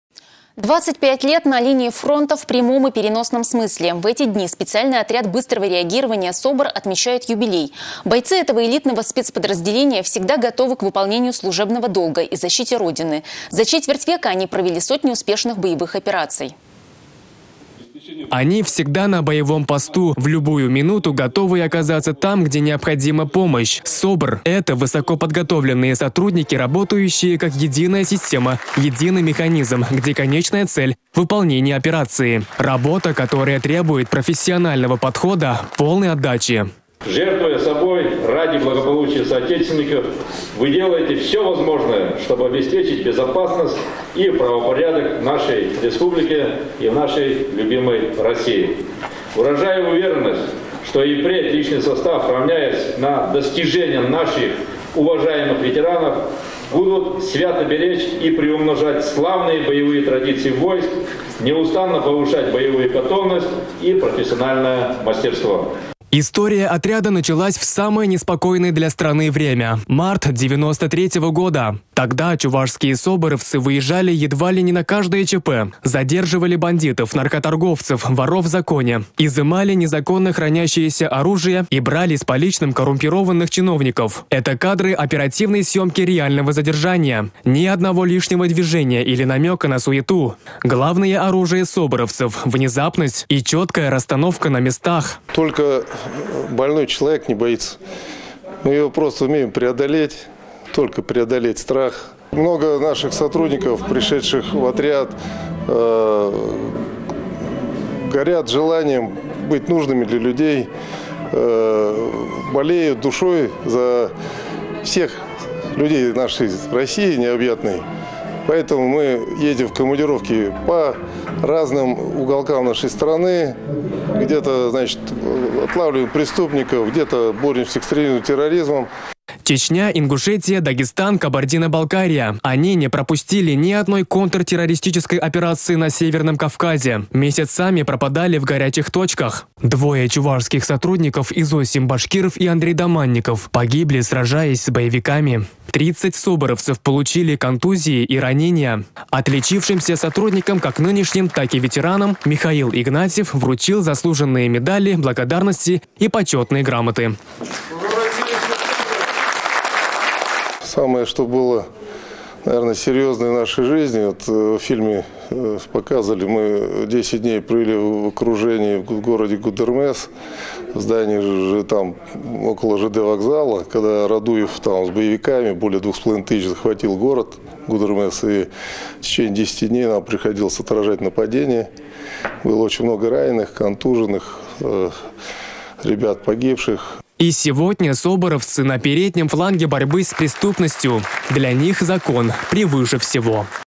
В эти дни специальный отряд быстрого реагирования - СОБР - отмечает юбилей.
Бойцы этого элитного спецподразделения всегда готовы к выполнению служебного долга и защите Родины. За четверть века они провели сотни успешных боевых операций.